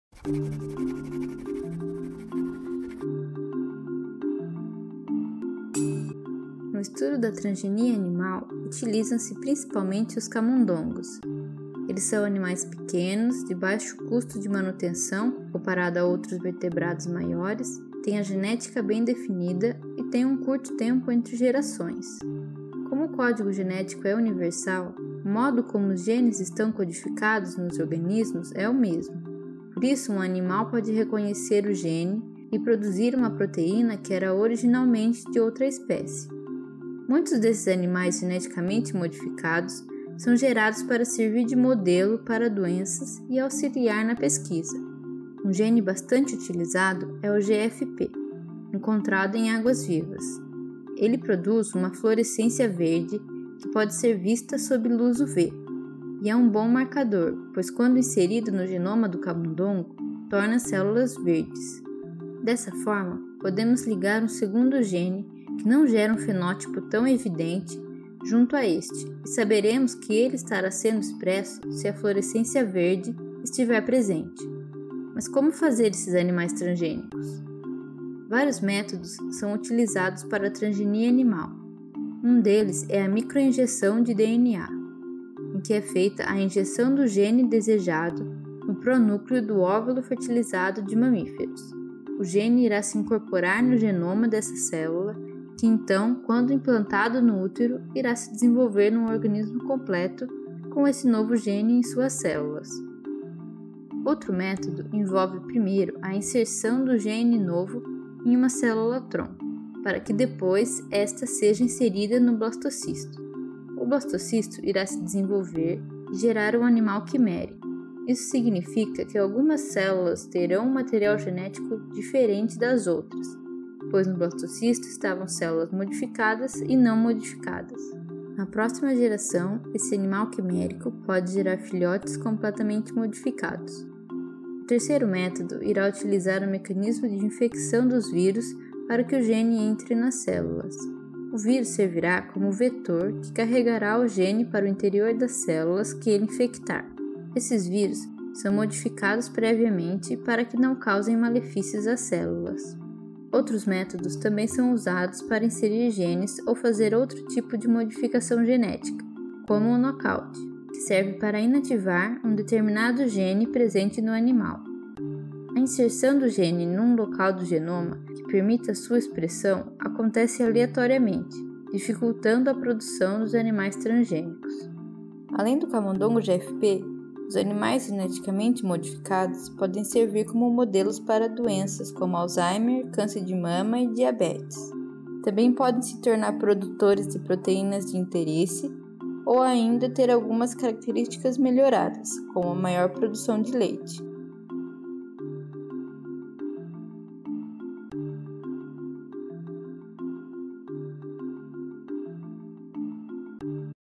Como são feitos os animais transgênicos? | InstanteBiotec 07
A transgenia em animais é um pouco diferente das plantas. Confira como fazer um camundongo fluorescente e para que servem esses animais geneticamente modificados.

Referências:
- Buy, M. Transgenic Animals. CCAC Resource Supplement, 1997.
- The University of Waikato. Making a Transgenic Cow. Biotechnology Learning Hub, 2010.
- Ikawa M, Kominami K, Yoshimura Y, Tanaka K, Nishimune Y, Okabe M. Green fluorescent protein as a marker in transgenic mice. Development, Growth & Differentiation, 37(4):455-459, 1995.
- Houdebine LM. Methods to Generate Transgenic Animals. In: Engelhard M, Hagen K, Boysen M. Genetic Engineering in Livestock: Ethics of Science and Technology Assessment. Berlin, Springer, 2009.
- Elder GA, Gama Sosa MA, De Gasperi R. Transgenic mouse models of Alzheimerl’s disease. Mt Sinai J Med, 77(1):69-81, 2010.
- Hutchinson JN, Muller WJ. Transgenic mouse models of human breast cancer. Oncogene, 19(53):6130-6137, 2000.